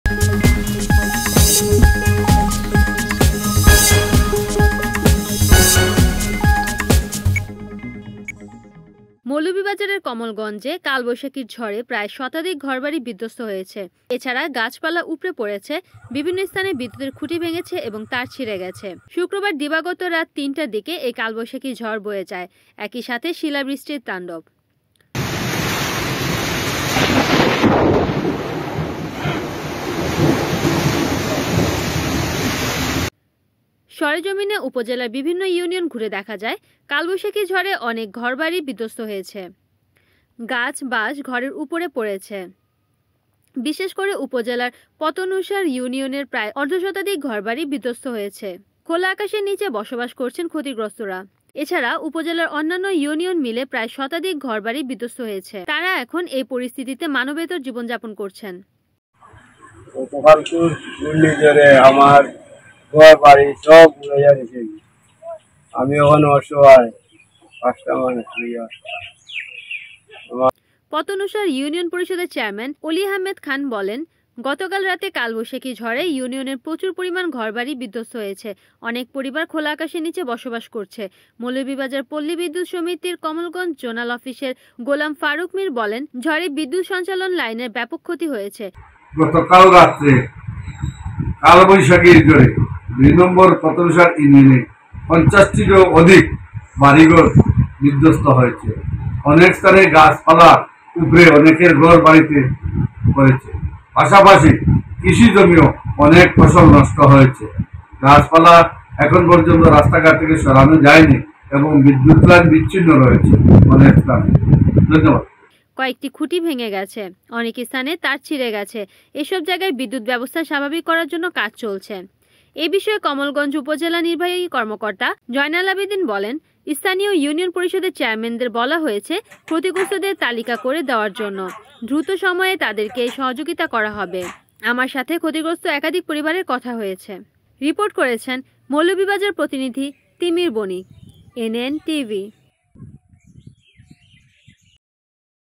কালবৈশাখীর তাণ্ডবে খোলা আকাশের নিচে শতাধিক পরিবার || NAN TV
কালবৈশাখীর তাণ্ডবে বাড়িঘর বিধ্ব*স্ত; আকাশের নীচে শতাধিক পরিবার || NAN TV

Official Address:
76/2 Uttor Chashara, Rajdhani Apurbo Tower,